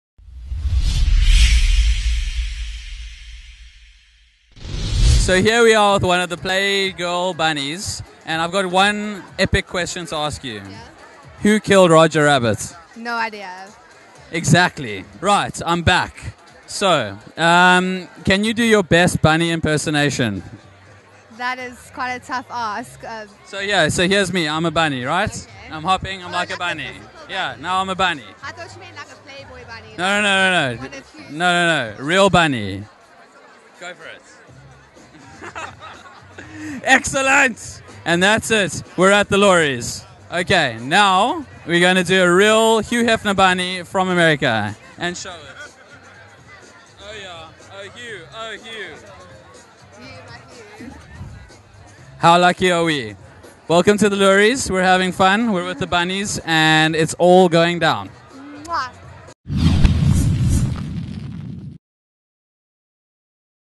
The real bunny
Jared Speaking to one of the Playboy bunnies